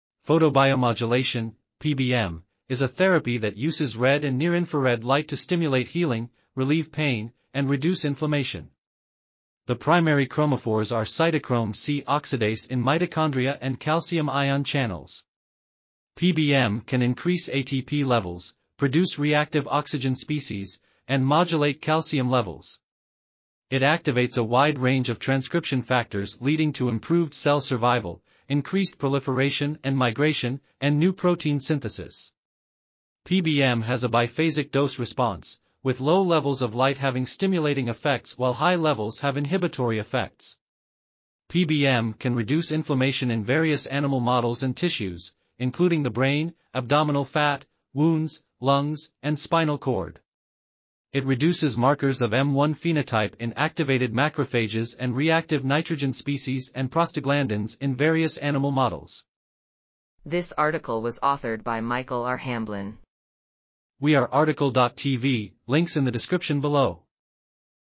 Mechanisms and applications of the anti-inflammatory effects of photobiomodulation | RTCL.TV
### Keywords ###

### Article Attribution ###
Title: Mechanisms and applications of the anti-inflammatory effects of photobiomodulation
Authors: Michael R Hamblin
Publisher: AIMS Press
DOI: 10.3934/biophy.2017.3.337

### Video Timestamps ###
0:00:00 - Summary
0:01:00 - Title
0:01:03 - Tail
0:01:07 - End